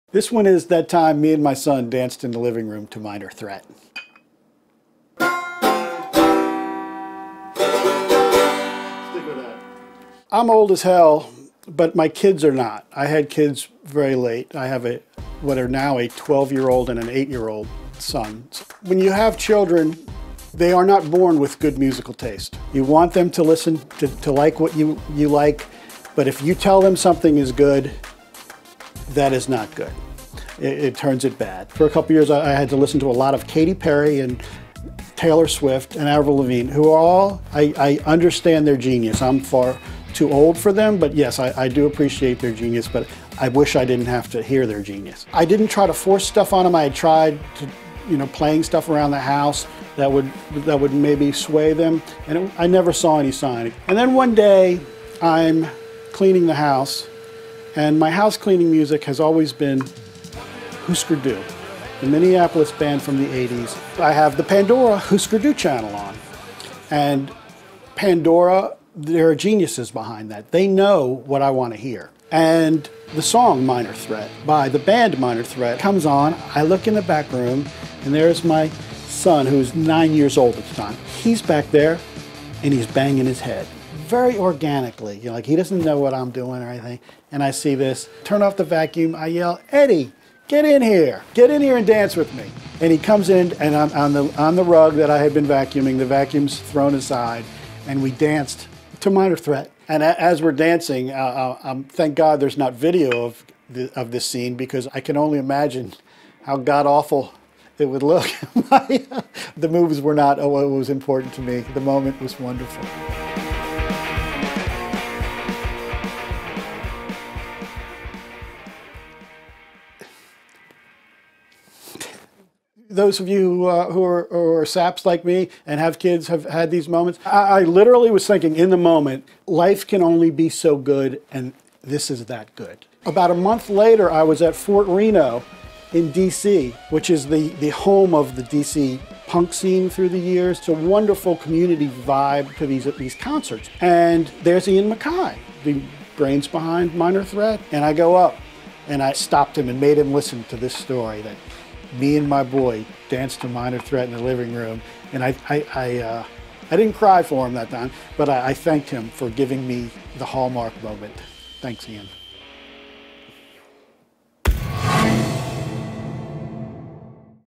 The Time Dave McKenna and Son Danced to Minor Threat
Deadpsin's Dave McKenna tells a heartwarming story about the time he bonded with his 9-year-old son over 1980s D.C. punk band Minor Threat.